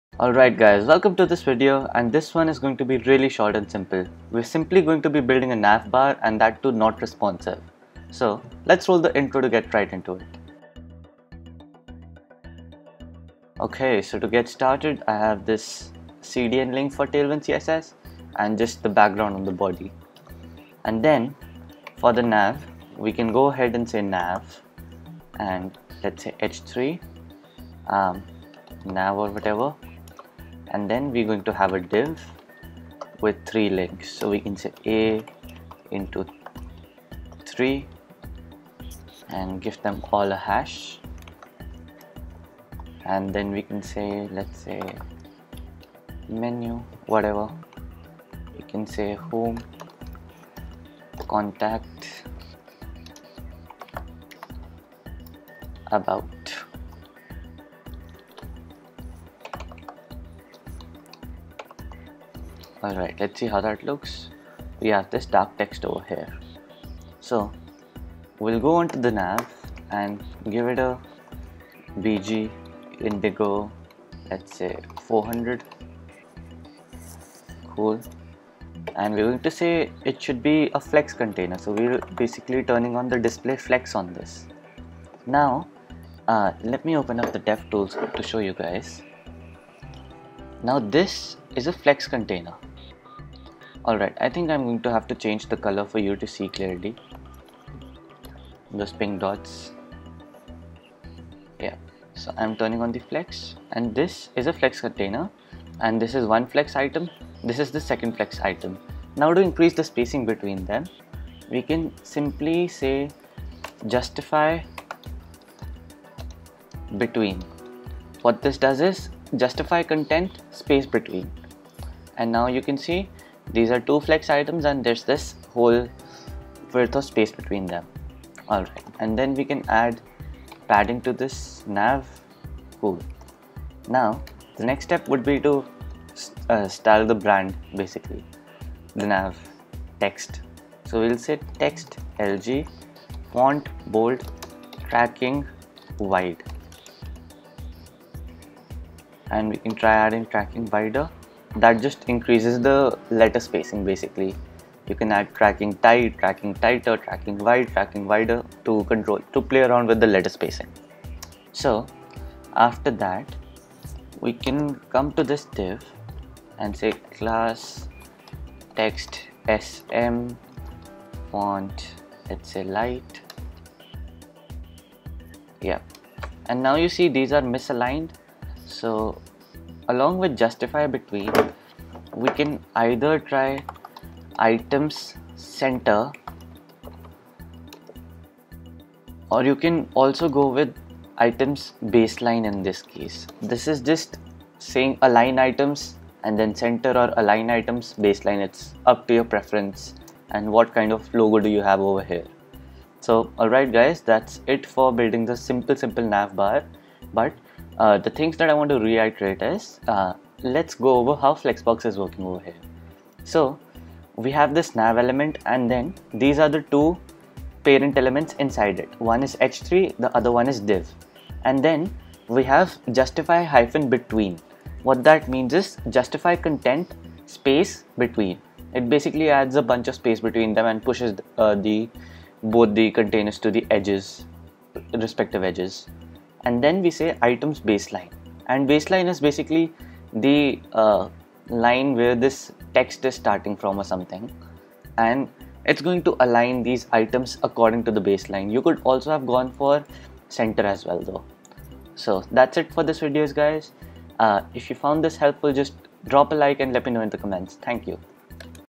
Navbar in tailwindcss, EP - 14 | Learn Tailwindcss
In this one we gonna build a navbar with tailwind!
I hope you guys liked this short and sweet video.